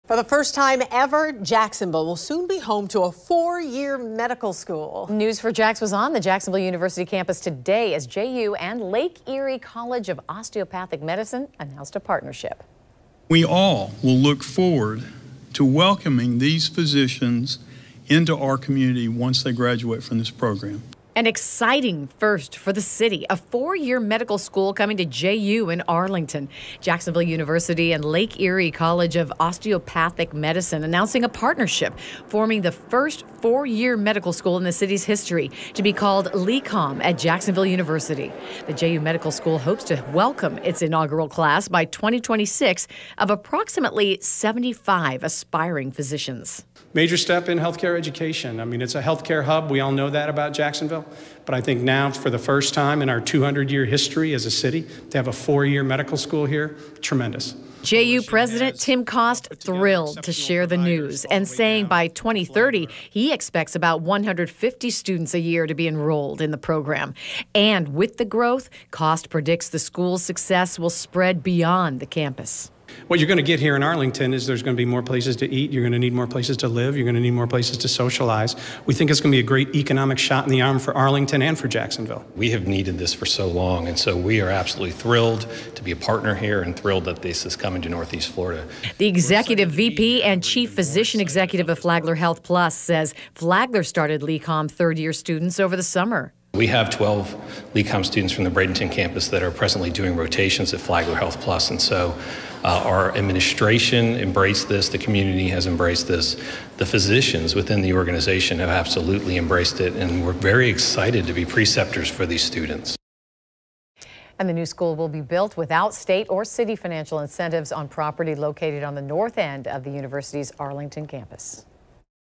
Jacksonville University announces area’s first 4-year medical school
On Wednesday morning, Jacksonville University and Lake Erie College of Osteopathic Medicine announced the partnership.